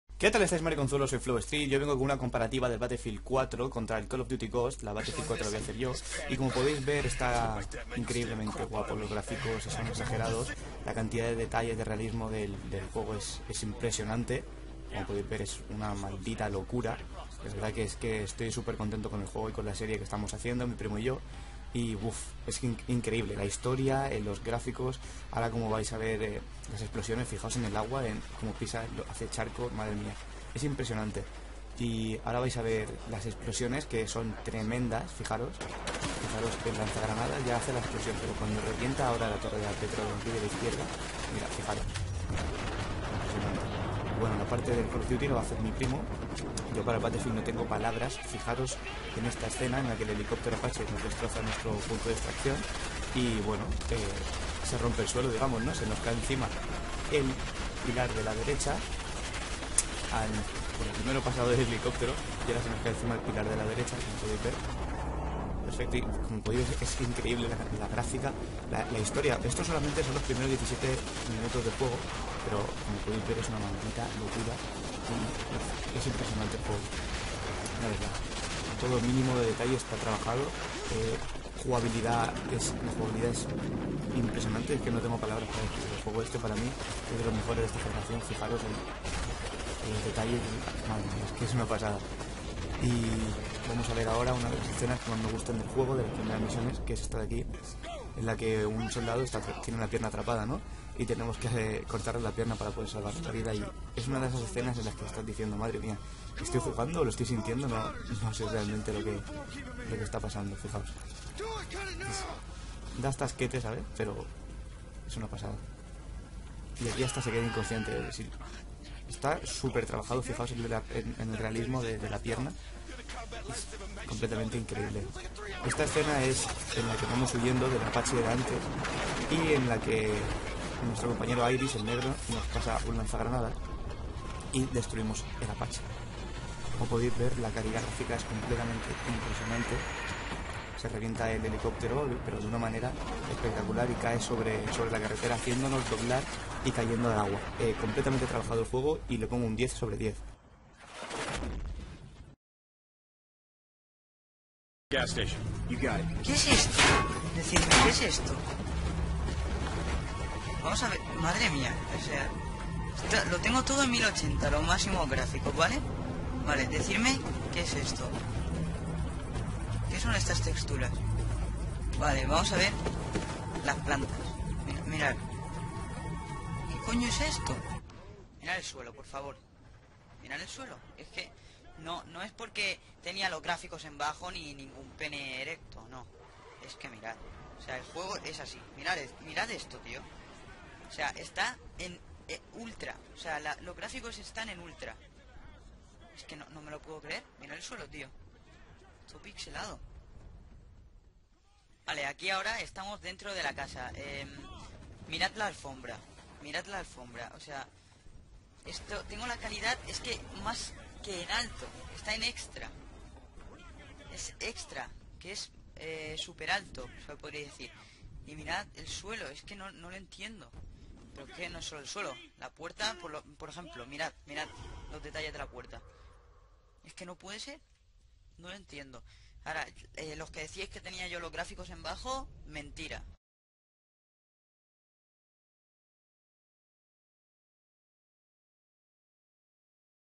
BATTLEFIELD 4 VS COD: GHOSTS | ¿Cual me compro?
Aqui os dejo una comprativa de Battlefield 4 contra COD Ghosts, hablado en español

Mi TWITTER: @FlowstreetYT